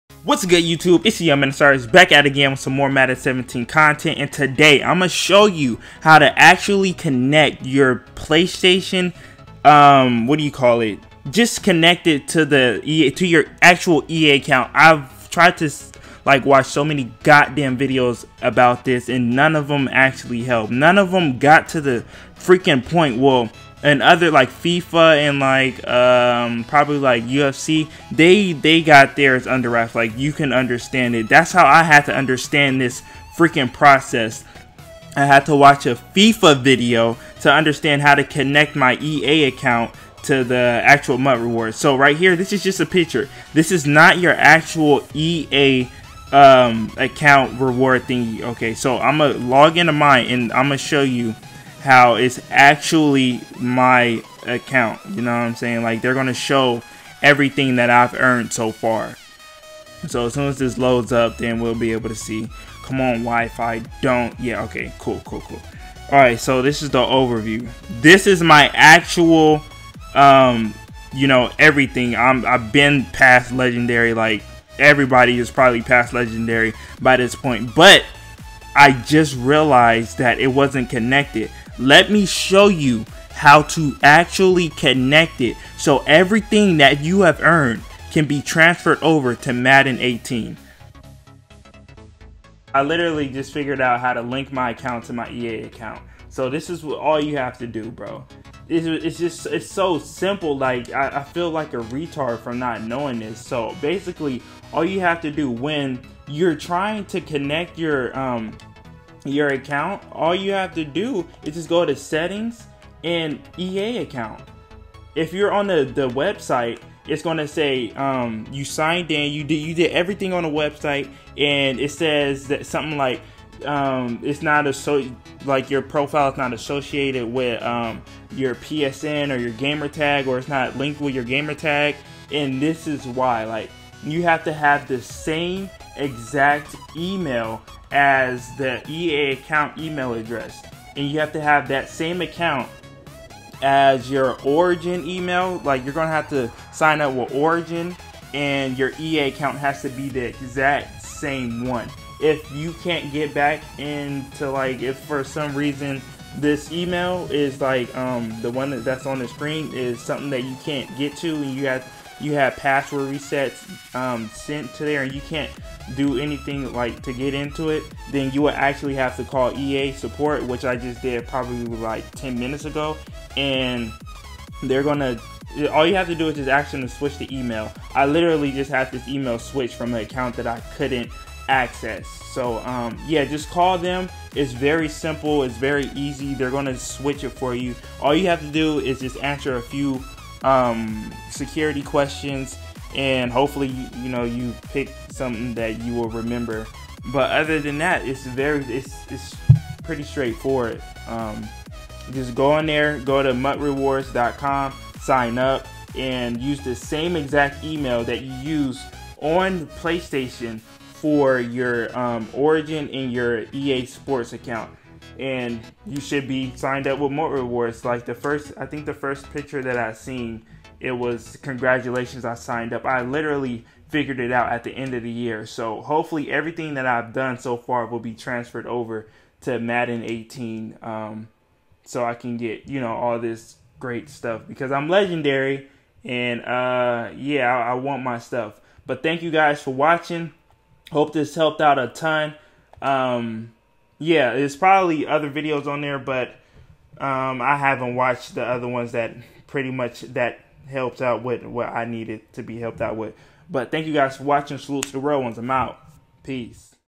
How To Connect EA Account To PS4/XBOX1 || MUT REWARDS , FUT REWARDS
How To Connect EA Account To PS4/XBOX1 || MUT REWARDS , FUT REWARDS. In this video i explain how to connect your console to your ea account. you may think you are signed in to mut rewards but may not be. go to easports.com and then find "contact us".